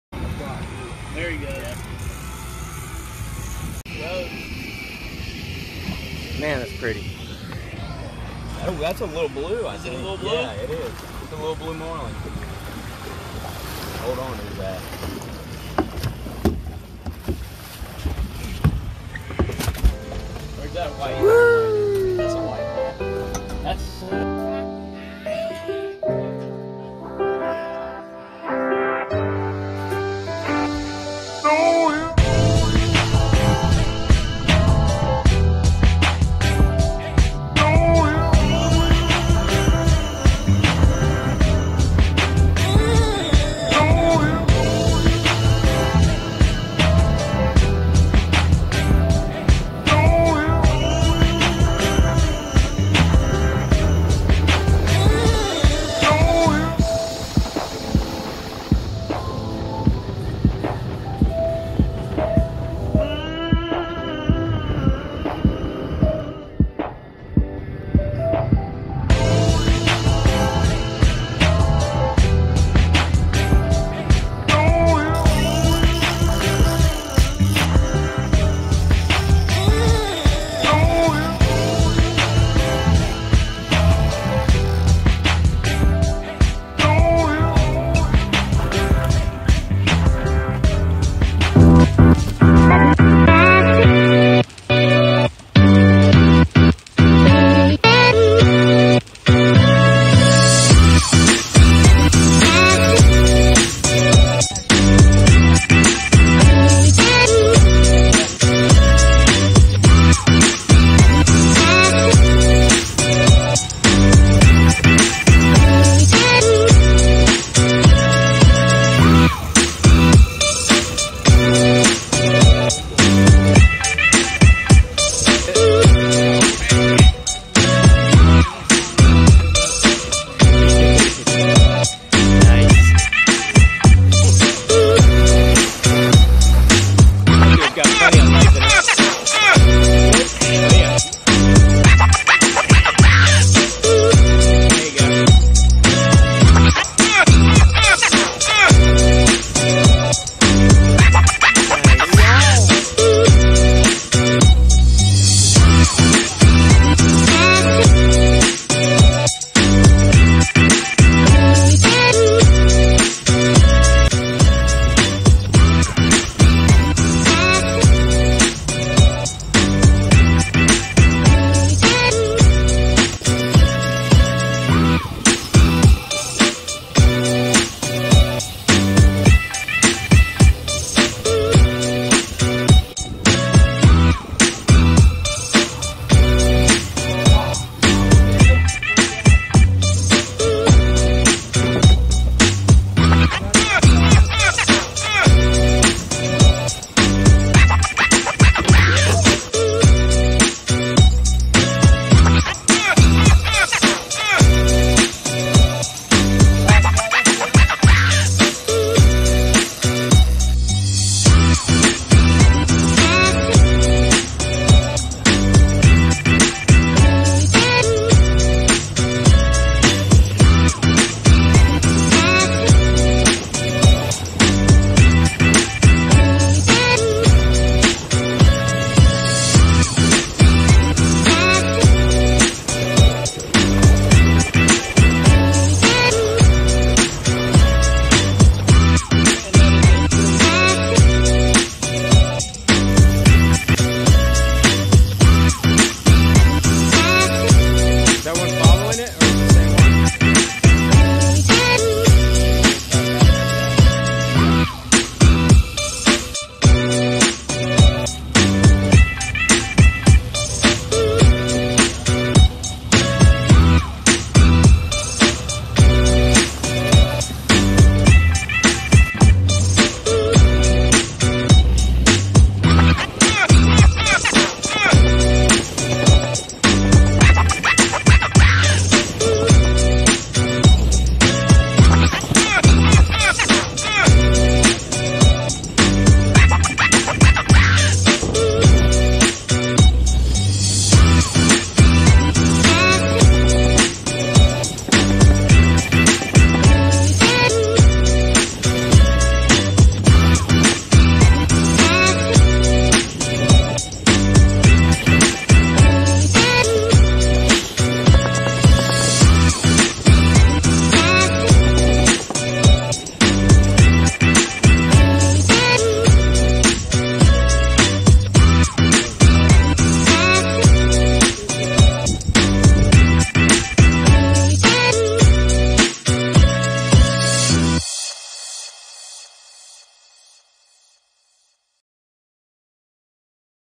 67 miles offshore, 1000ft of water, and a long patch of Sargasso Grass
Another day, another cooler full of fish and a lifetime of memories, with Capt. Robbie (@diggsrob) of @releasemesportfishing aboard his @contenderboatsofficial that we coated with @feynlab Hydro.

Accompanied by @capt_bh, and @tommylewis

Watch the video I pieced together, then book a trip. Yes, I fish too, I help bring in lines, and I help swab the deck for the next batch of fish to be landed. So there are chances to get awesome footage that I do miss.

And visit these other fine pages